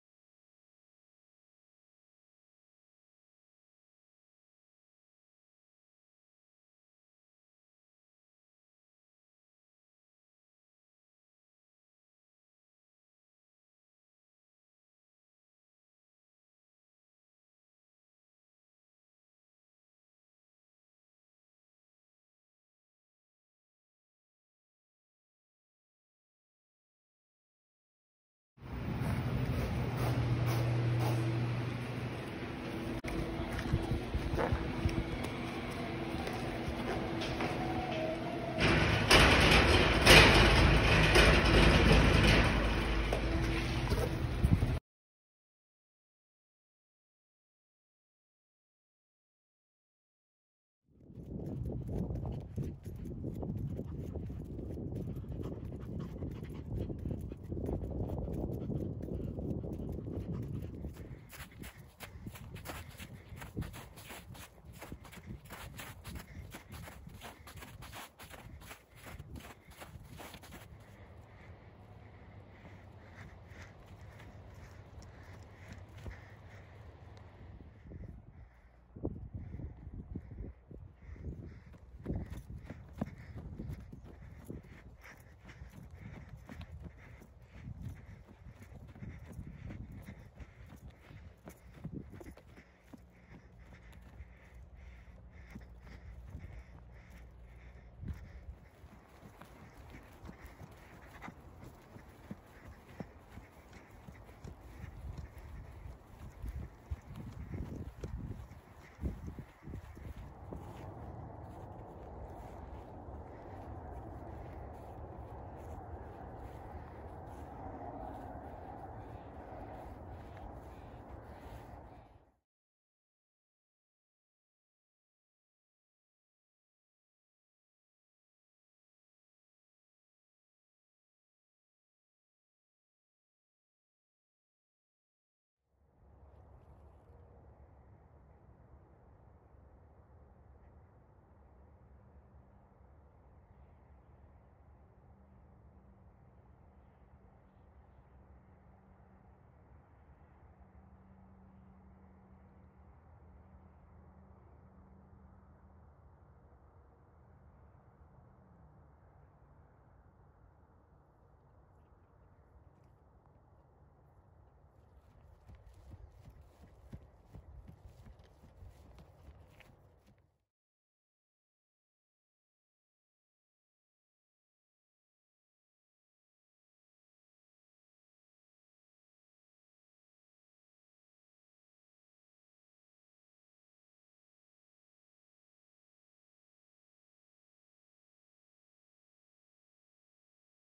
Rescued Chimpanzee Runs Around Outside
Burrito has taken a liking to running around Young's Hill with his caregivers this past year. No matter the true season, it's running season for Burrito.